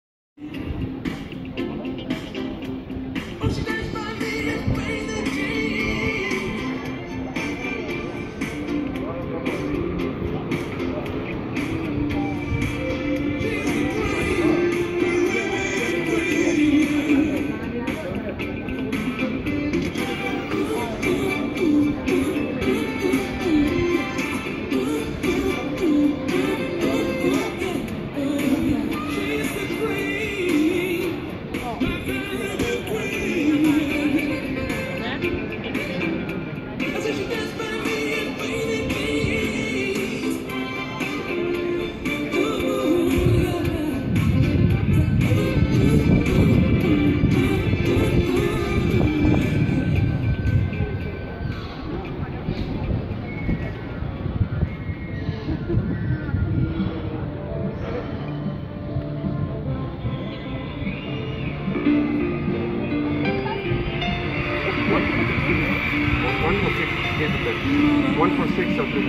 Fabian Marcel Gaete - Finger painter in 1 minute
Meet him in Malaga at Calle Sta Maria, we saw him here this Summer,